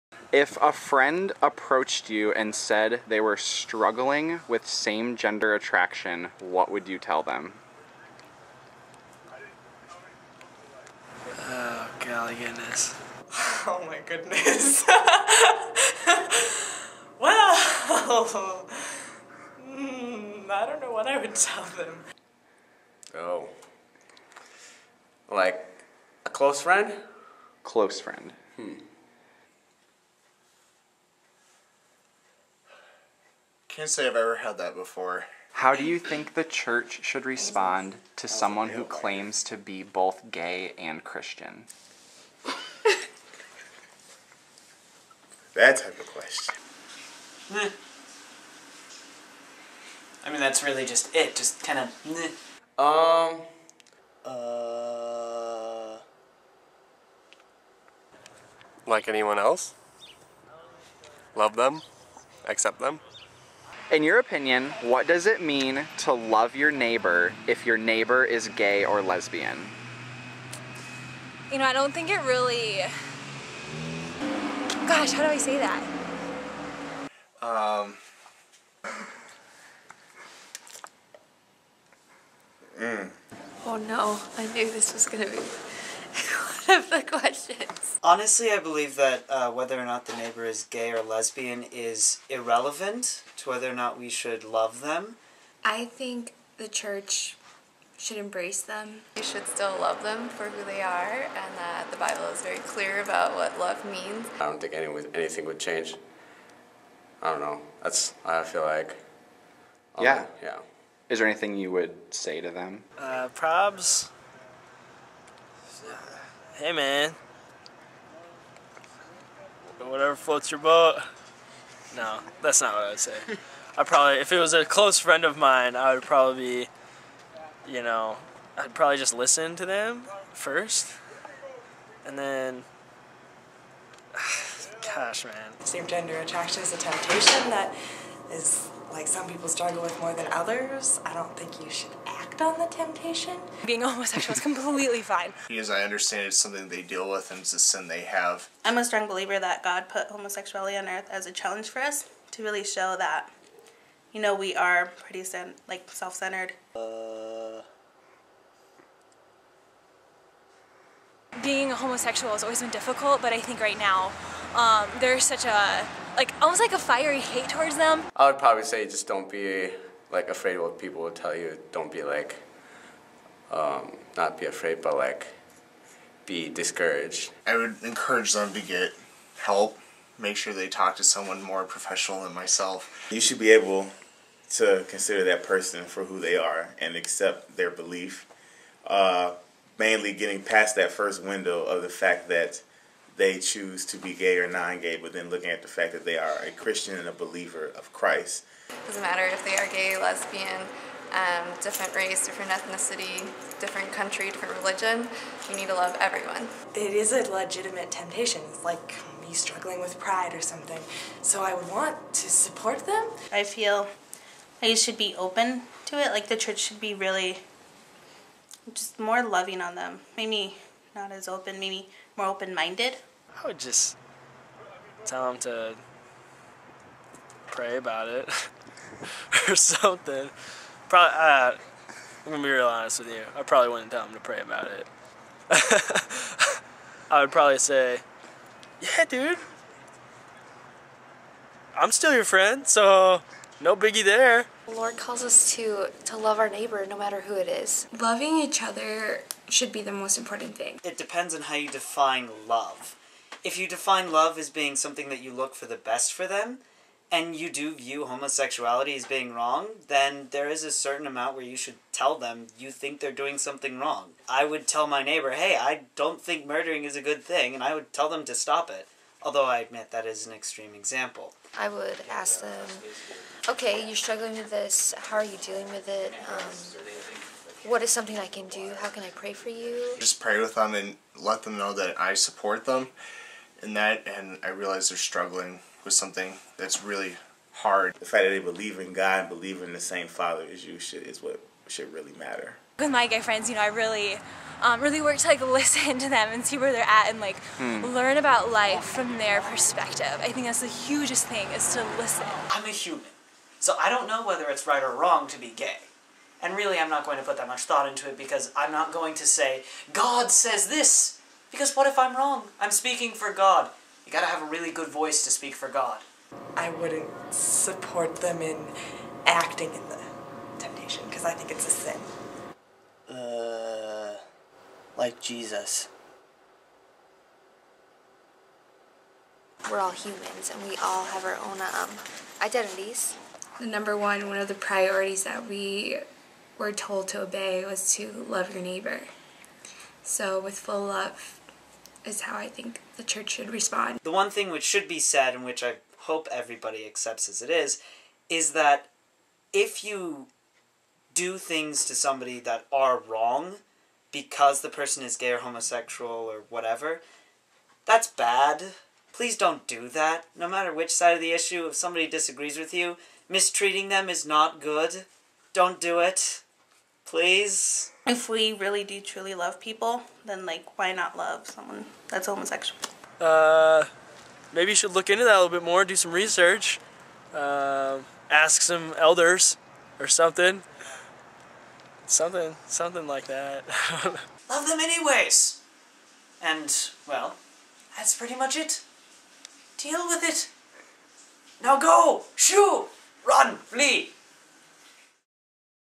Elevate the Conversation
This is the video that was shown to set the stage for a special chapel service at Northwestern College in Orange City, Iowa on April 5, 2013. My friend (Taylor) and I went around our campus earlier that week in an attempt to see how students think the Church should respond to our LGBT neighbors. We were surprised by the wide spectrum of answers we received reminding us that Christians are not arriving at the same conclusions.

My hope with this video is that we elevate the conversations that are already happening. For example, let's avoid yes or no questions, but instead ask questions like, "What does it look like to love my neighbor if my neighbor is LGBT?" "How would I respond to a friend who claims to experience same gender attraction?" "Why do you think churches are resistant to become inclusive of all people?" We have important questions to ask as we seek to fully live out the two greatest commandments Jesus gave his followers. What if Christians were known for what they were for instead of for what they are against? Let's do this, Church. Let's be known by our love.

If you yourself are ready to elevate the conversation, feel free to start by sharing this video. I feel like it has the potential to be a great conversation starter: a new kind of conversation that so desperately needs to happen.

**And please remember, the views expressed in this video do not necessarily represent the views of Northwestern College**